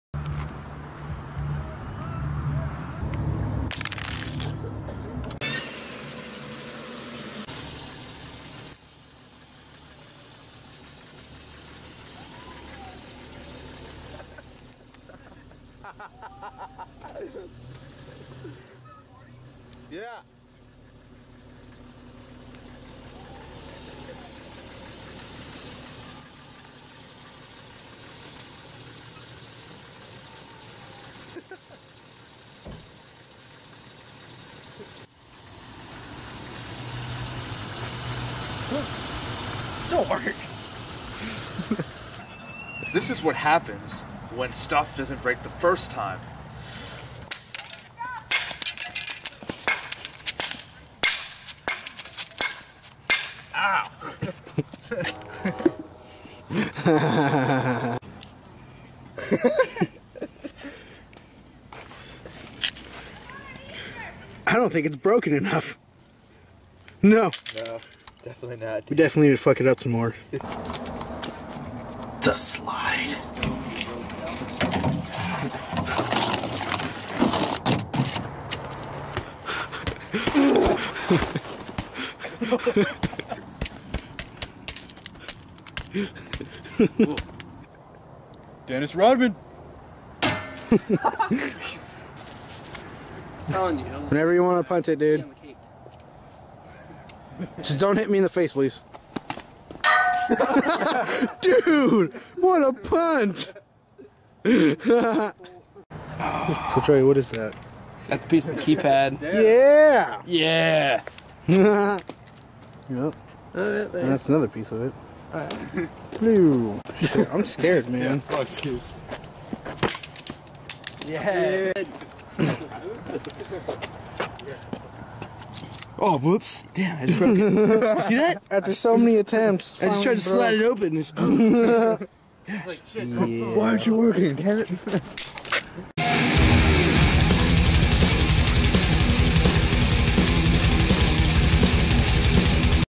Cell Phone Destruction
HectikHavok fucking shit up once again, this time it is Hectik's worthless broken cellular phone that stopped working